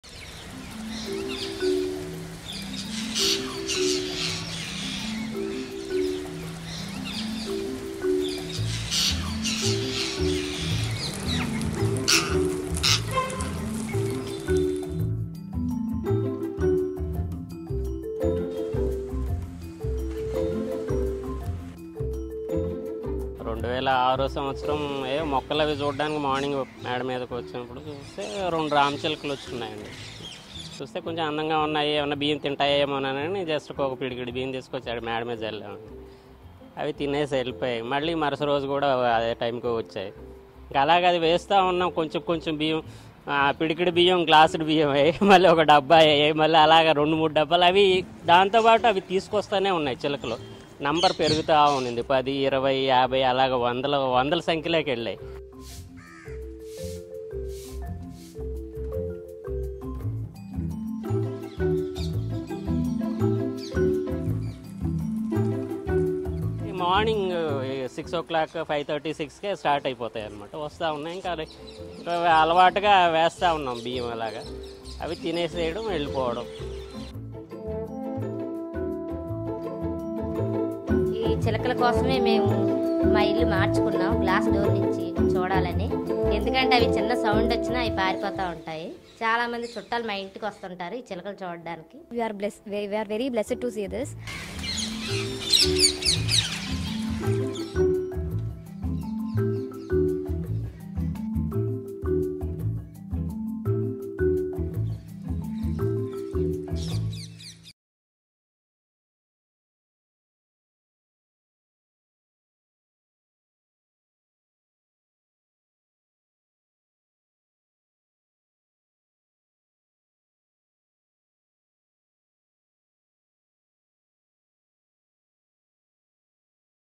Why hundreds of parrots visit this same terrace every day - BBC News
Lakshmi Narayan Reddy's home in the southern Indian city of Visakhapatnam has become a popular spot for the city's ring-necked parakeets.

Hundreds of them flock to his terrace three time a day, where he sprinkles grains of rice for them to peck.

It's now become a daily routine and one that he has perfected over the last 14 years - much to the delight of his neighbours who often peek out of their windows and watch as the parrots gather on his terrace every day.

Video by BBC Telugu's Lokkoju Srinivas